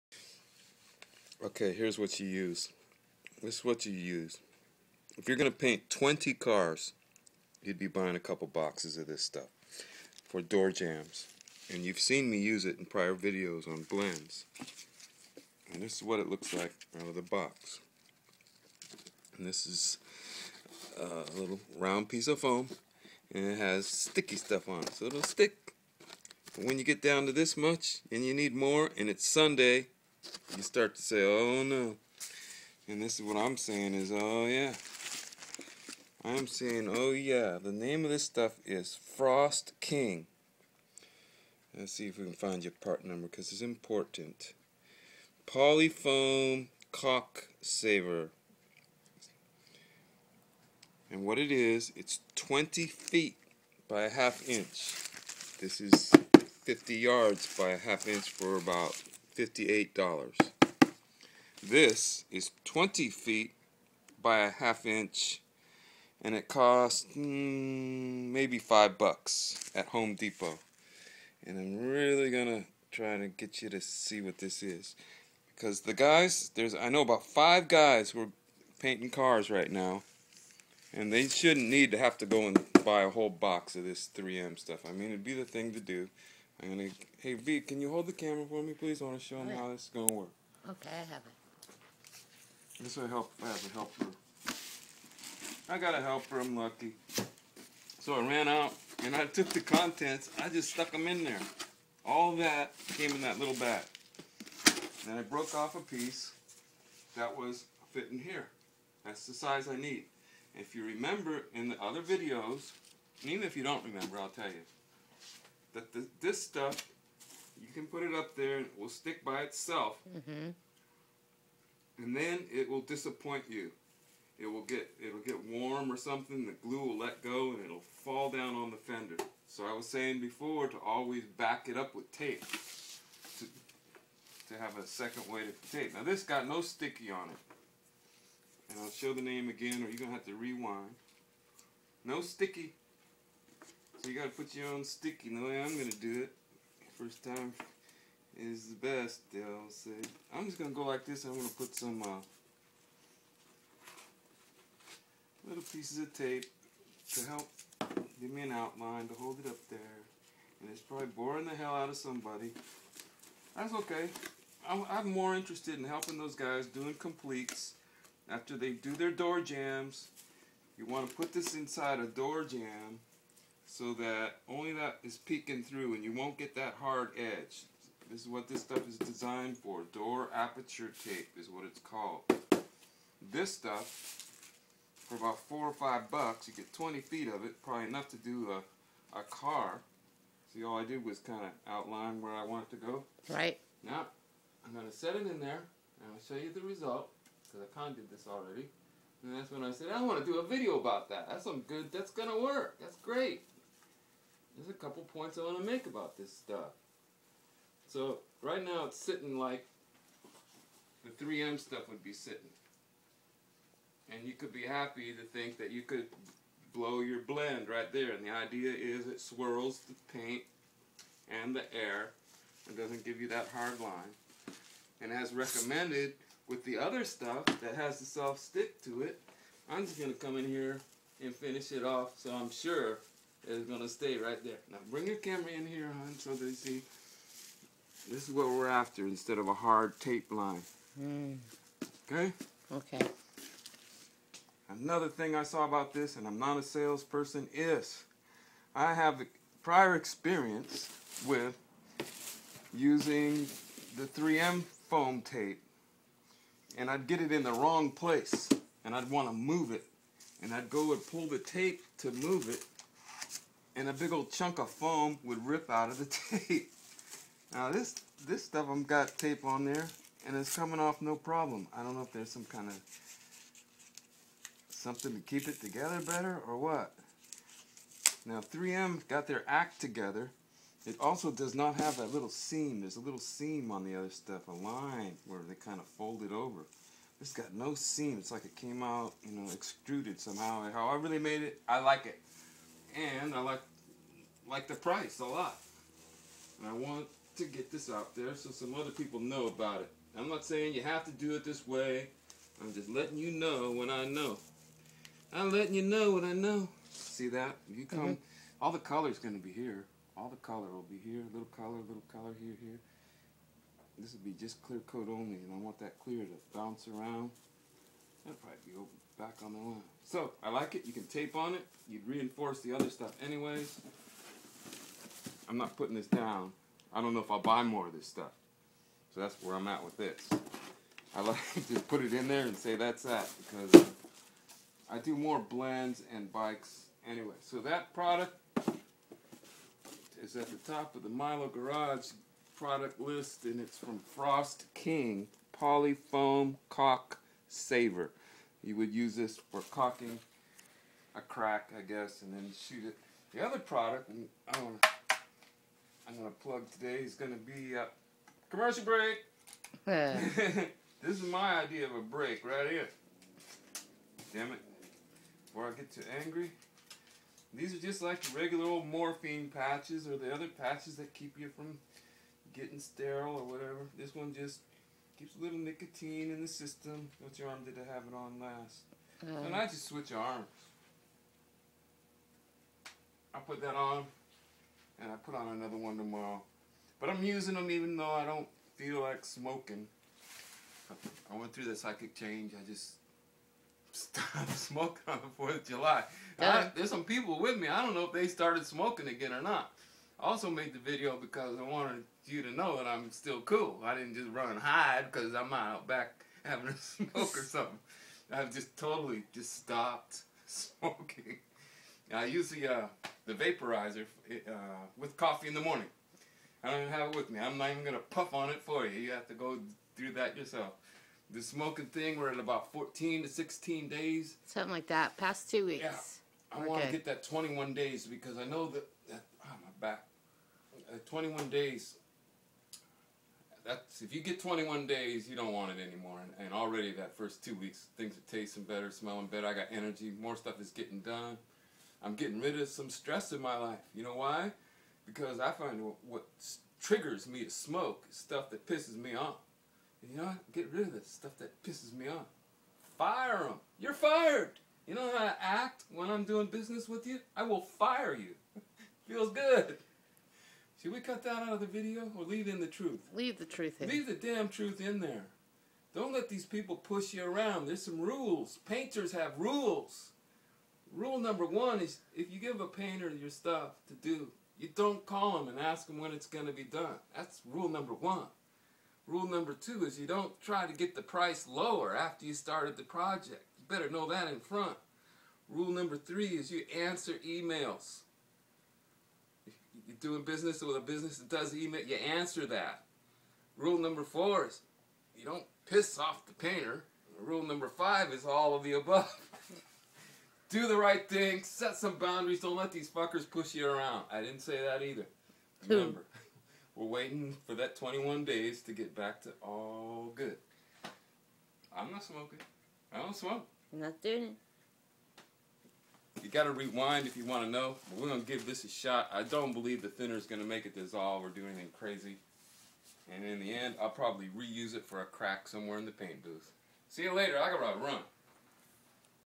How to mask door jambs for blends and overspray
When painting a car a foam tape is used as to not leave a "hard" edge on freshly painted door jambs, deck litds ,under the hood ,gas lids etc. The 3M stuff is ok but will dissapoint you from time to time.